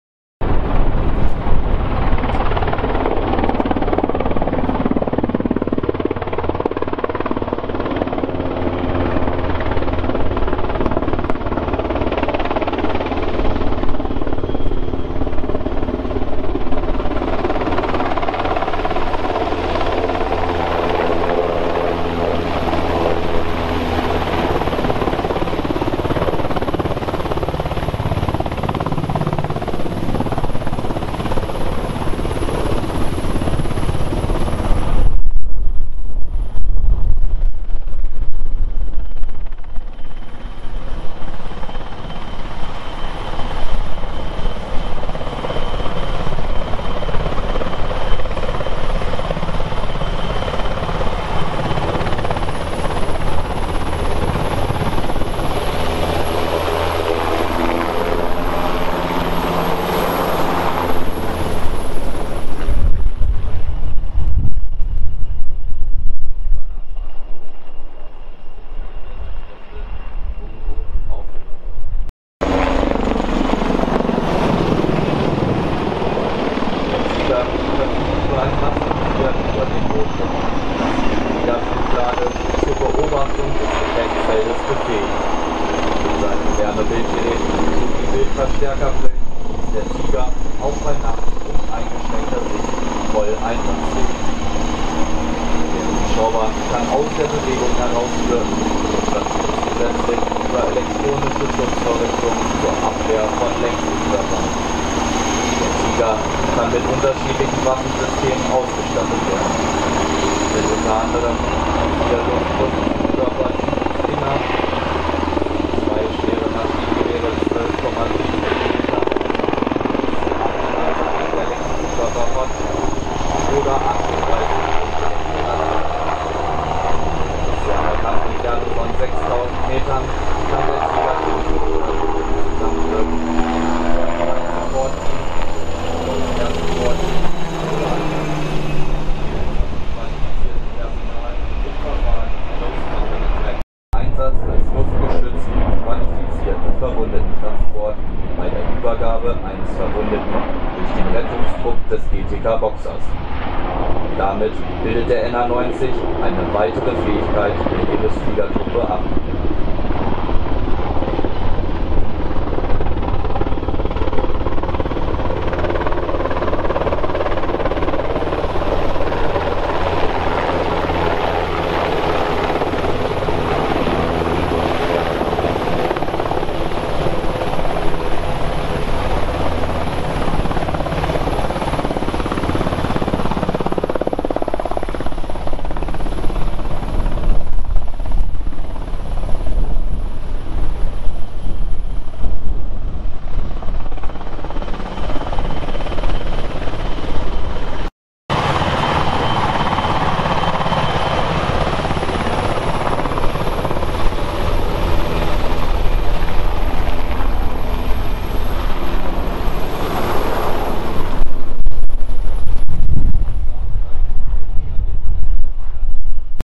ILÜ 2019- NH 90 und Tiger
NH 90 und Tiger- Informationslehrübung Landoperationen der Bundeswehr am 11.Oktober 2019 auf dem TrÜbPl  Munster.